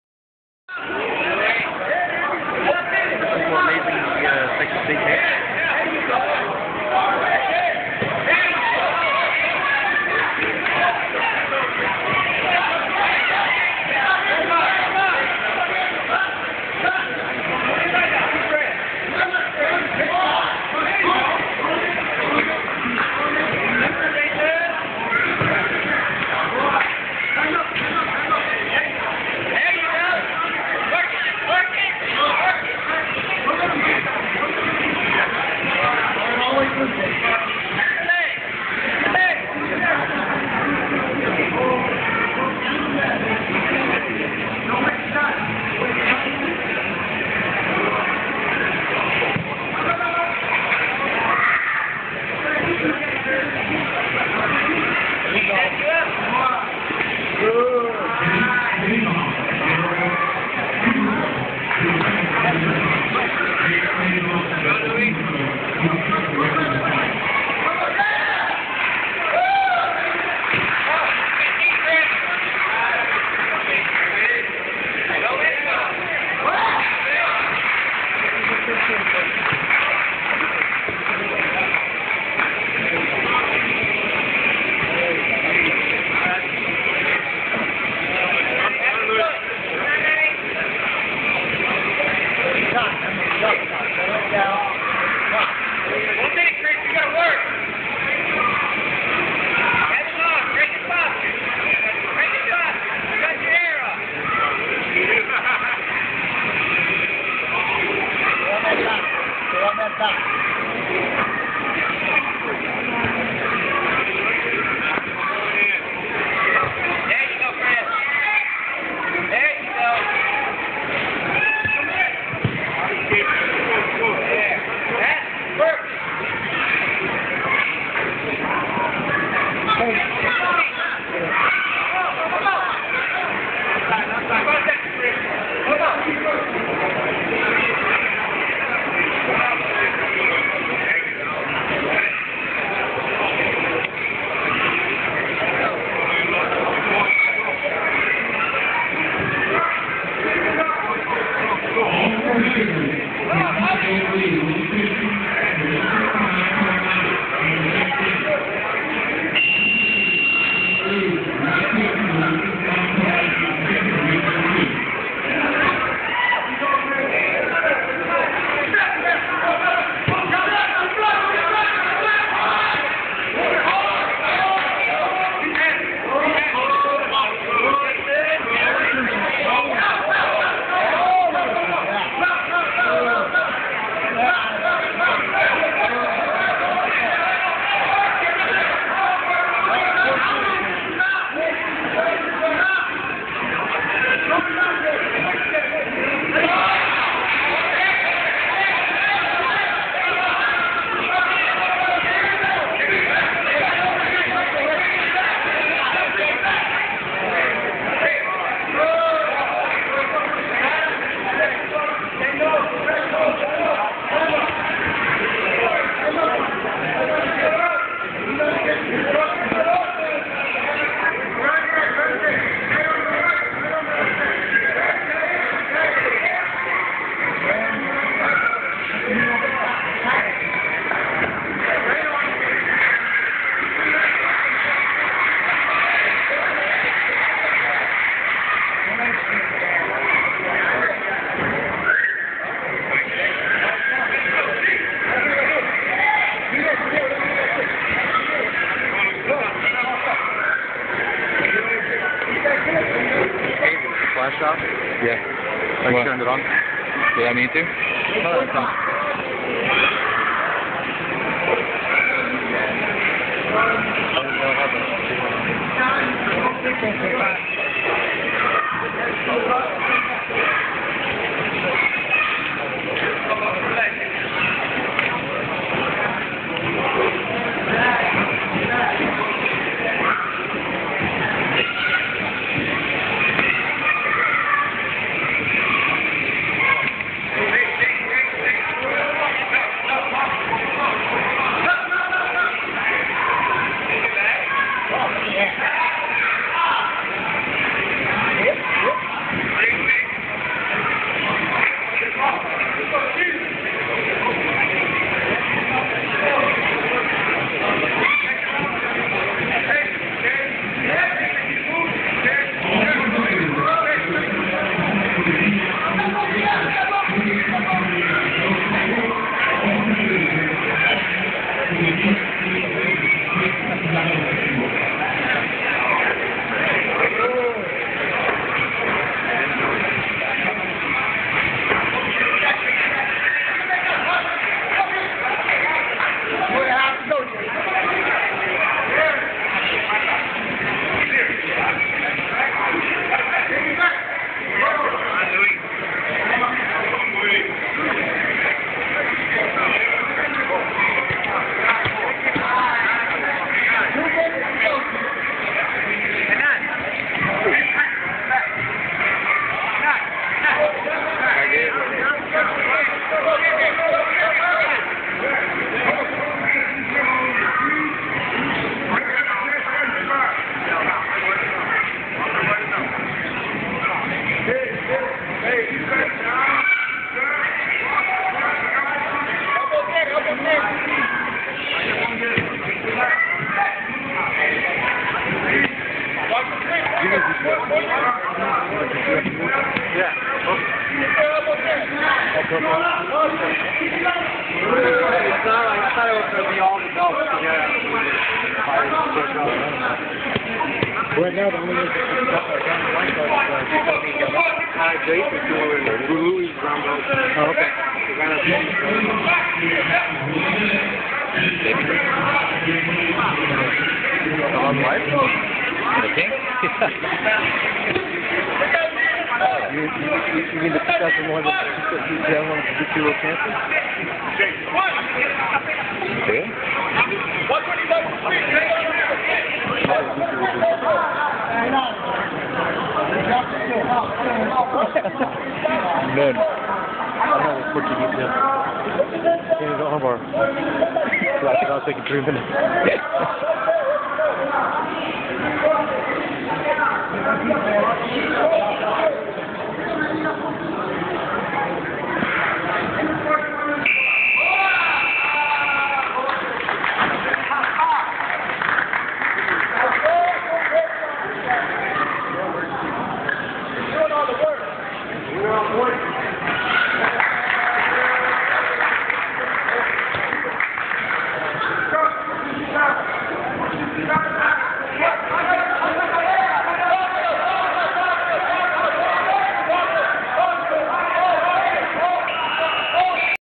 Jason Soliz vs Luis Rubalcava - F4F Tourney by TXMMA.com
This match was recorded by Mike Calimbas at the 'Fight for Faith' BJJ Tournament in Houston on June 19, 2011.